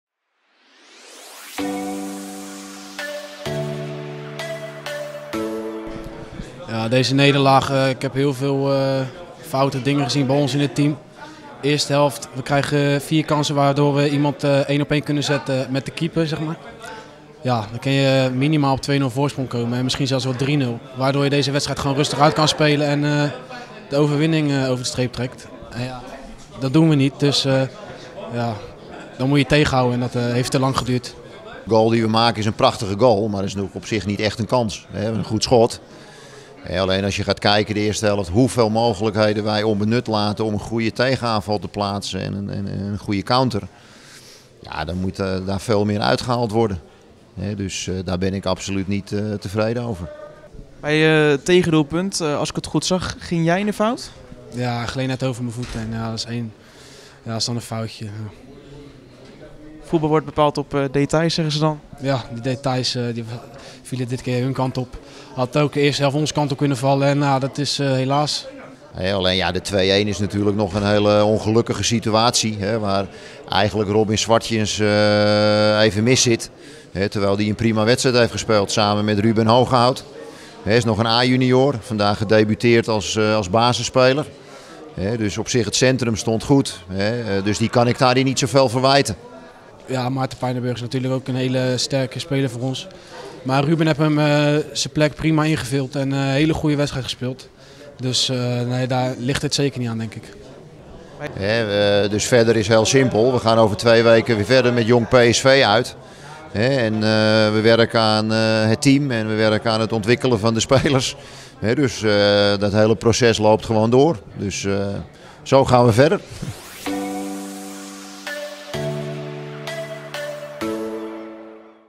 'Nu valt die bal fraai binnen'
Trainer/coach Robin Pronk en aanvoerder Robin Zwartjens van Jong FC Utrecht blikken terug op de wedstrijd tegen FC Eindhoven (1-2).